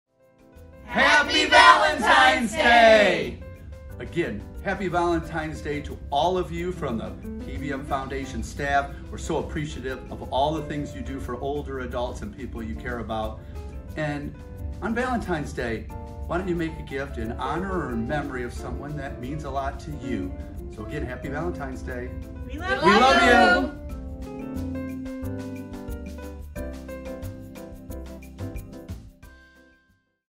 💖 Happy Valentines Day from PVM Foundation!
Happy Valentine's Day from all of us at the PVM Foundation!
In the spirit of love and remembrance, we invite you to make a gift in honor or memory of someone dear to you. Each contribution is a testament to the lasting bonds that shape our lives and fuel our mission.

Join other compassionate supporters by making a gift in honor or memory of someone special in your life. Your contribution today will not only pay tribute to a loved one but also help us continue our vital work, ensuring that the legacy of your loved ones helps to build a brighter future for those we serve.

Thank you for considering a gift that not only honors a loved one but also makes a meaningful difference in the lives of others. Together, we can continue to spread love and compassion throughout our community.